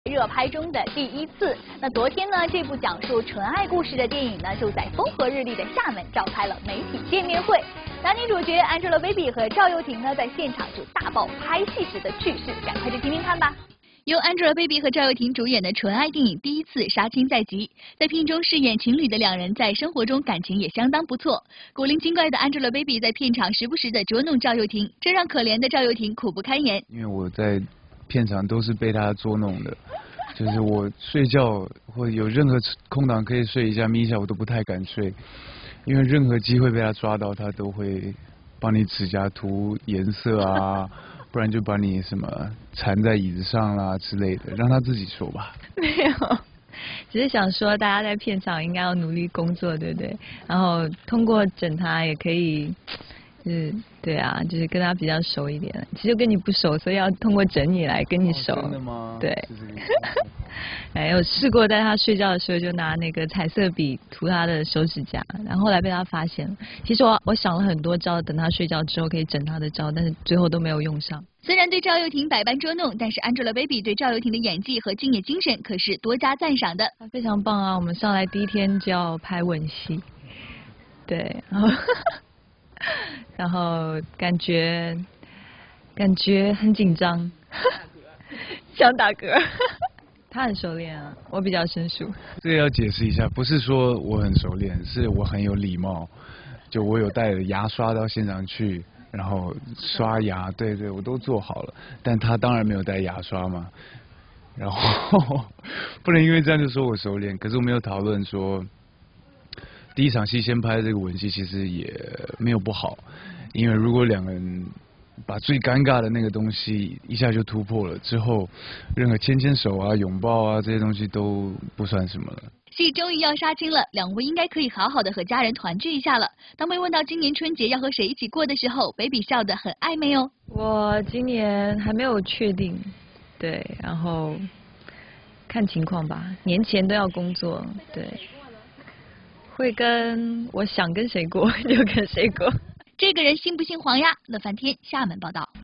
《第一次》片場 楊穎調皮 捉弄趙又廷
電影《第一次》由北京數字印象文化傳播有限公司、安樂影片有限公司、萬誘引力電影聯合出品，江志强監製，80後導演韓延執導。1月8日主演趙又廷、Angelababy、江珊在厦門與當地媒體見面，暢聊在當地拍攝的各種趣事。